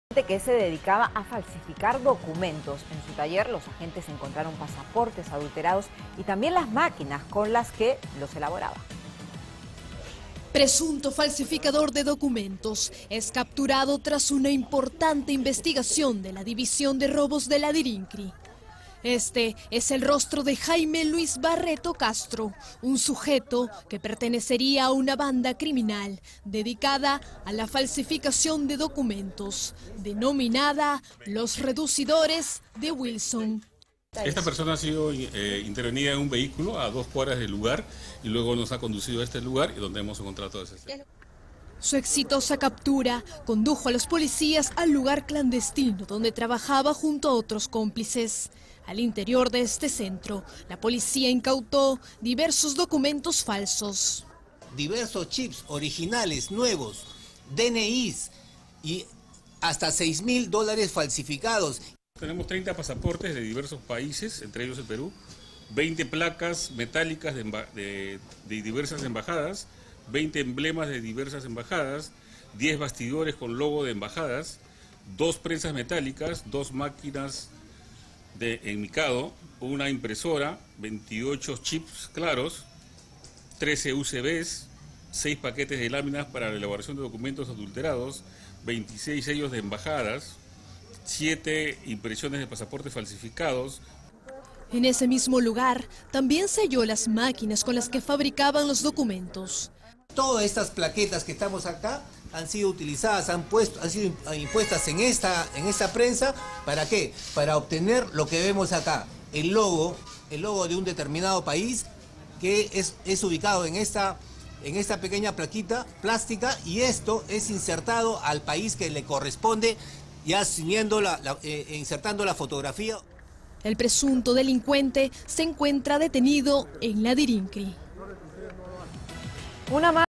Detienen a sujeto acusado de falsificar pasaportes: tenía chips, DNIs y 6 mil dólares falsos (2/2)
Un presunto falsificador de documentos fue capturado tras una importante investigación de la División de Robos de la Dirincri.

El sujeto fue identificado como Jaime Luis Barreto Castro, quien pertenecería a una banda criminal dedicada a la falsificación de documentos denominada Los Reducidores de Wilson.

Su exitosa captura condujo a los policías al lugar clandestino donde trabajaba junto a otros cómplices. Al interior de este centro, efectivos incautaron diversos documentos falsos, chips originales, DNIs hasta 6 mil dólares falsificados.

El presunto delincuente se encuentra detenido en la Dirincri.